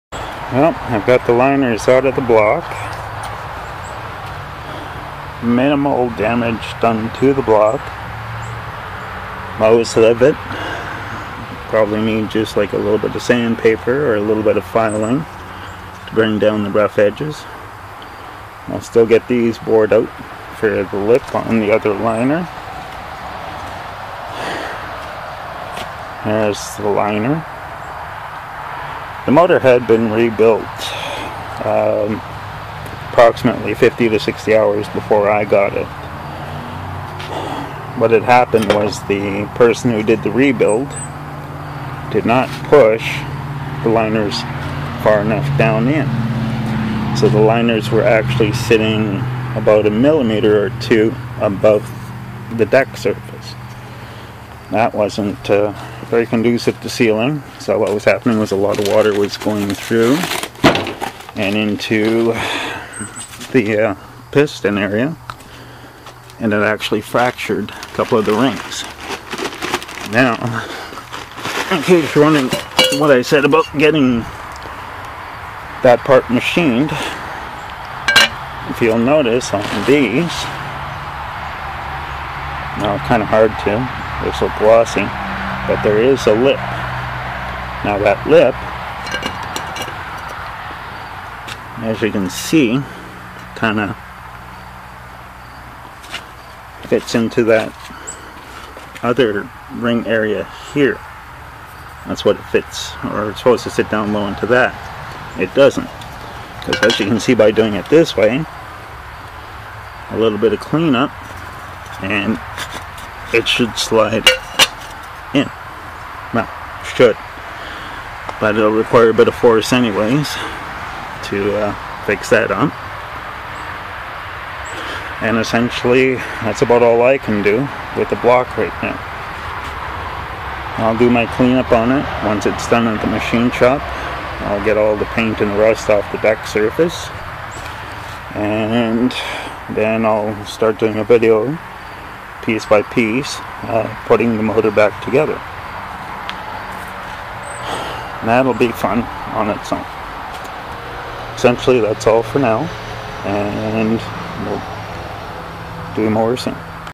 Cylinder Liner / Sleeve Removal How To with No Budget. Part 2
Sorry about the blurry video. The camera had trouble focusing. I had a budget to rebuild this engine and then stuff happens. The machine shop quoted me a very reasonable price to machine out the sleeves. The price was $30 Can per liner. I was all set to do the deal when crap happened so I had to compromise some. The block has to be machined to accept the new liner because of a lip on the new liners to help keep it in place. I needed to know how much machining because it looked like something was in that spot for the lip already. I got ambitious and devised a plan (hair-brained) and commenced to shatter the liners. Here it is. I used a flat edge screw driver and hammer.  I also used a pair of vice grips to hold the screw driver after I smacked my thumb twice with the hammer. And no I am not a red neck, I just think that way sometimes.